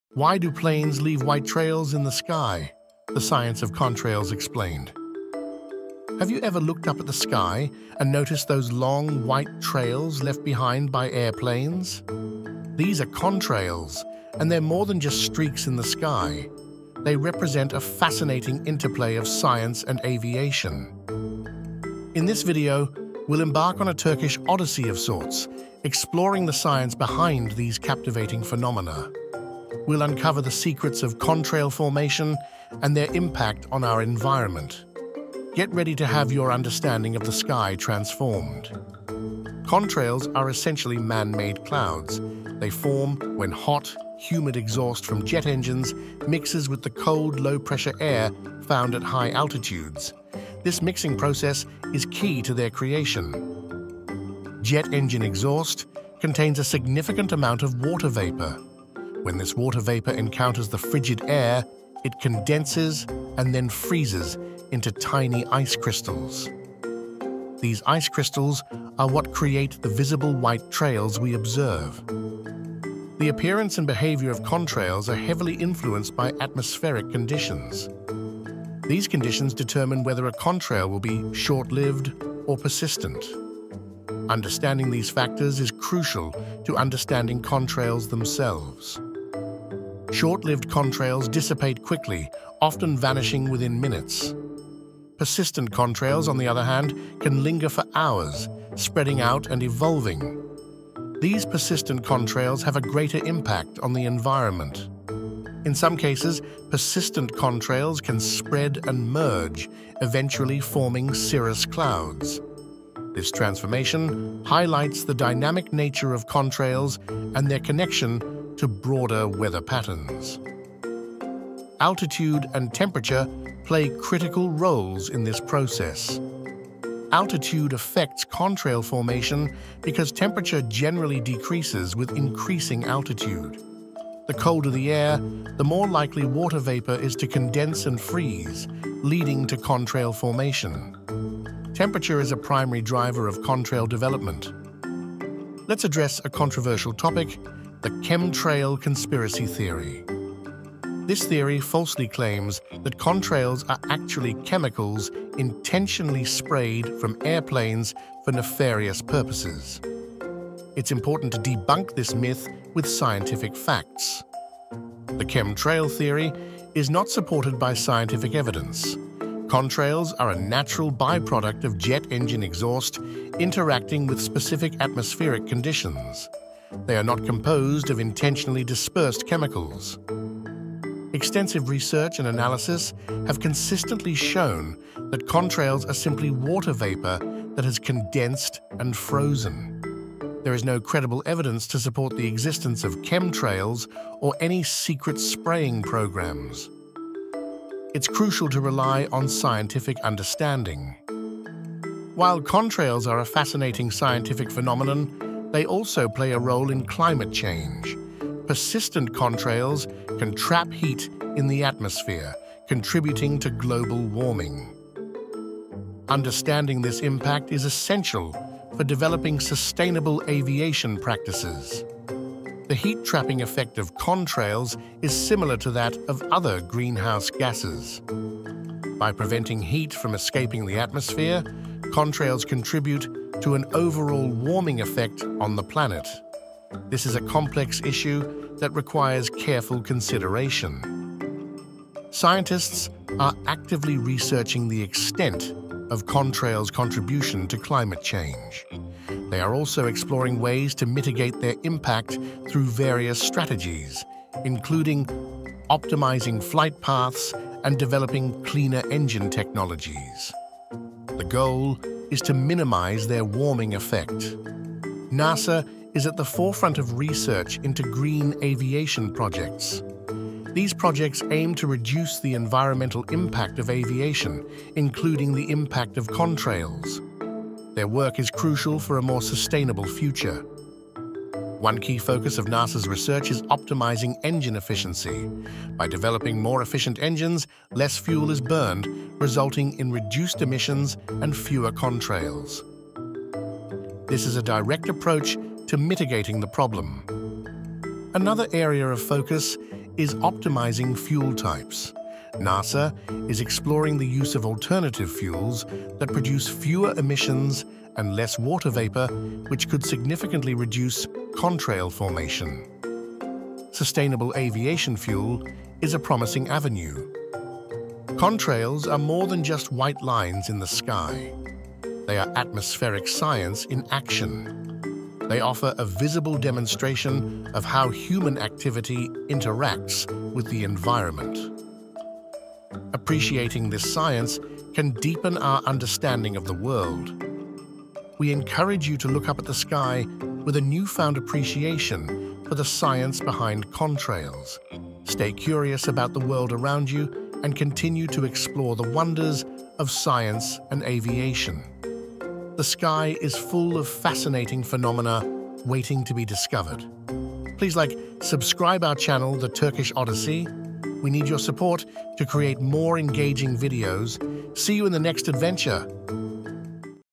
Why Do Planes Leave White Trails in the Sky?
Ever wondered why airplanes paint white streaks across the sky? These lines, known as contrails (short for condensation trails), are more than just pretty patterns-they're a fascinating intersection of aviation, atmospheric science, and even climate impact.

In this episode of The Turkish Odyssey, we break down:

What contrails are and how they form ☁️✈️

The different types: short-lived, persistent, and spreading

Their surprising role in global warming

Debunking the chemtrail conspiracy theories

How altitude, temperature, and jet engines shape these trails

What the future holds with NASA's green aviation projects 🌍

✨ Whether you're a science buff or a curious traveler, this deep dive will change how you look at the sky.